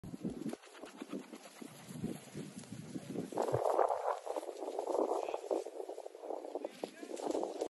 Fawn, Standard poodle puppy pheasant hunting training
Fawn, first day with live birds. Just shy of 5 months old.